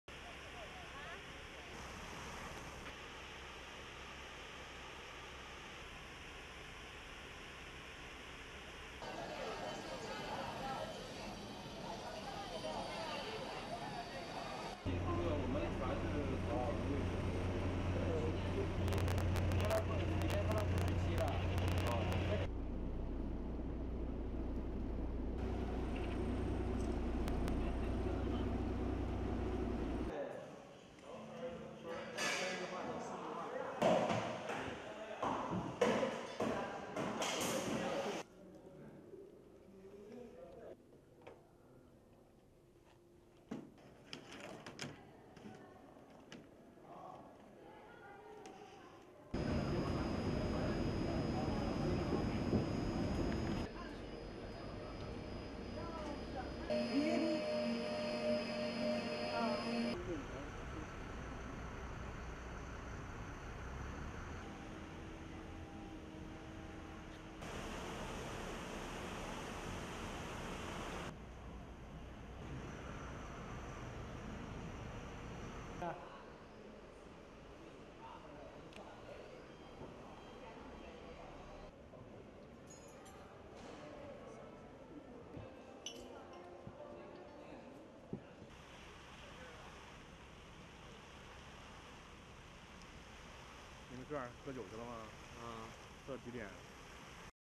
Busy Preparations Underway for Boao Forum for Asia
Busy preparations are underway for the annual Boao Forum for Asia (BFA) slated to open on Thursday in Boao town, south China's Hainan Province.